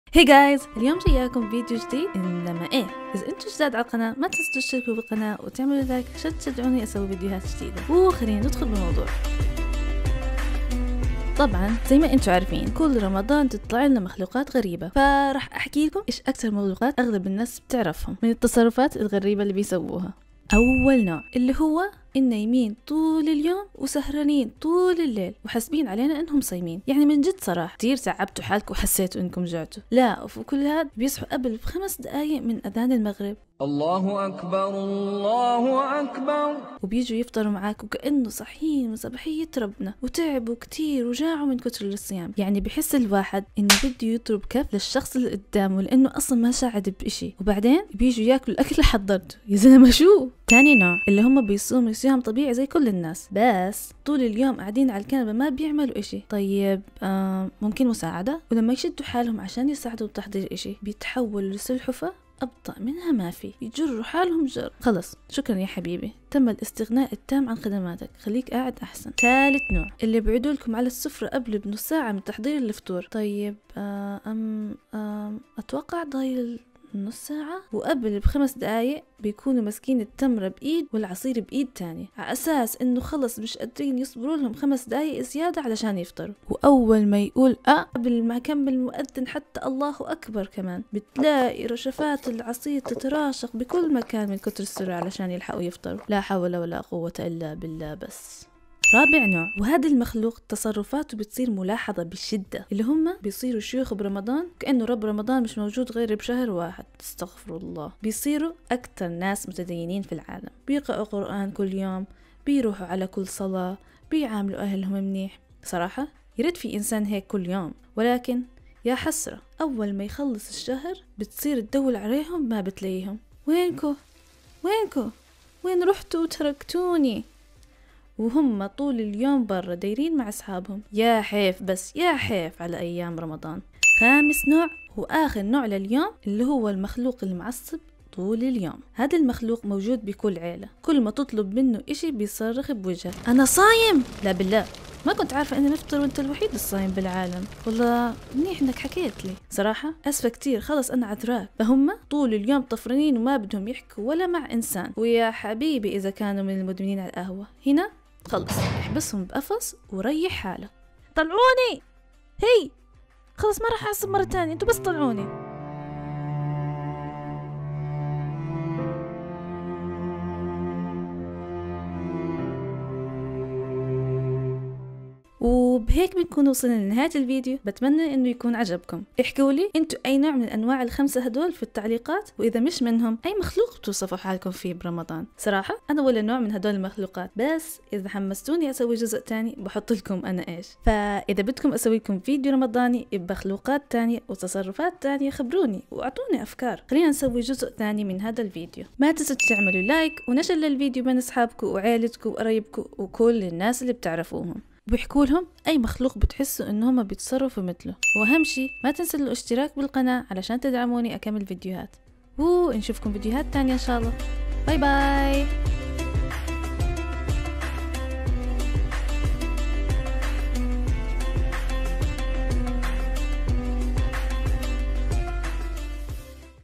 Ramadan Animation - أنواع الناس في رمضان!! أنيميشن
Hey guys!!
مرحباً جميعاً!!

Ramadan is right at the corner you guys!! can't wait for it to begin and eat all these delicious meals!!
رمضان كتيير قرب!! وكتيير متحمسة لكل الأكلات اللذيذه تبعته!

Today I came with an animation video!! wohooo!!
اليوم اجيتكم فيديو أنيميشن وأخيراً!!

I finally finished it! it took me soooo long!!
أخيراً خلصته! كتيير أخذ مني وقت صراحة!

It's about the types of people during Ramadan! and how they annoy us sometimes lol!!
هوه عن أنواع الناس في رمضان! وكيف بعض المرات بيكونوا شوووي مزعجين لول!

BUT!! I'll leave you with the video now! ENJOY!!
بسسس رح أخليكم مع الفيديو هلقيت! استمتعوا!!

وكمان اذا بدكم تسووا في فولو على السوشال ميديا!

Instagram and Facebook: Justtraneem

Bye bye!!
باي باي!

[ENGLISH SUB COMING SOON]